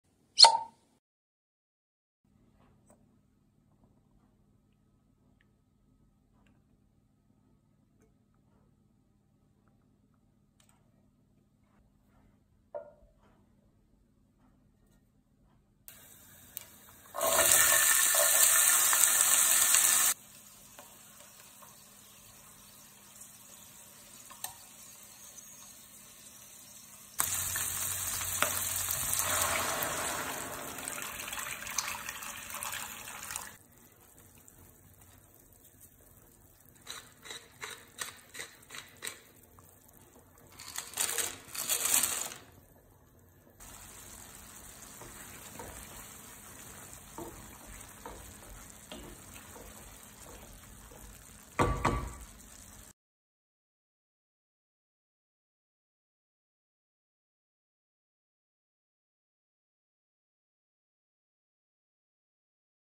Mafalde e ceci | Pasta with chickpeas | Cibo Vino | Food Wine
Ingrendienti:

Olio
Aglio
Rosmarino
Ceci lessati 80g
Sale
Pepe nero
Acqua 350g circa
Mafalde 120g

Ingredients:

Oil
Garlic
Rosemary
Boiled chickpeas 80g
Salt
Black pepper
Water approximately 350g
Pasta 120g